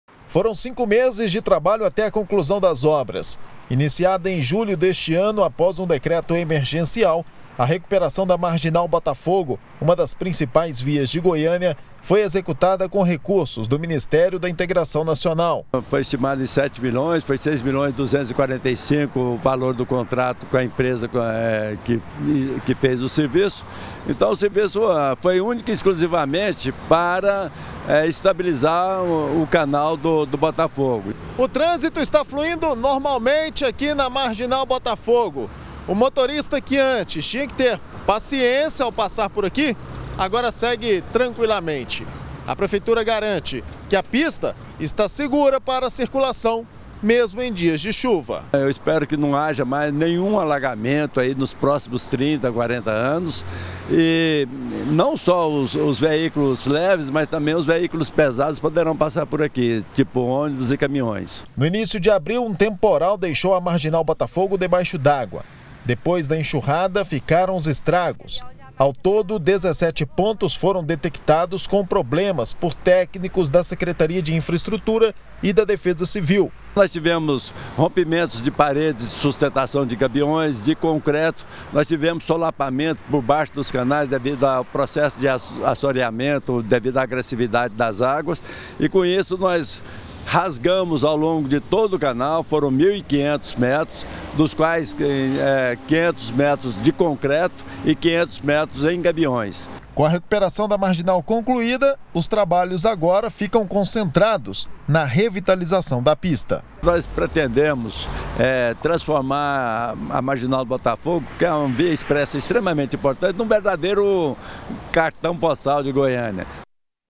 MARGINAL BOTAFOGO: PISTA É TOTALMENTE LIBERADA DEPOIS DE OBRAS
A Marginal Botafogo foi totalmente liberada depois das obras de reparos por causa de desabamentos. A pista expressa que liga Goiânia de Norte a Sul teve todo o seu canal restaurado. Ao todo, 17 pontos foram recuperados.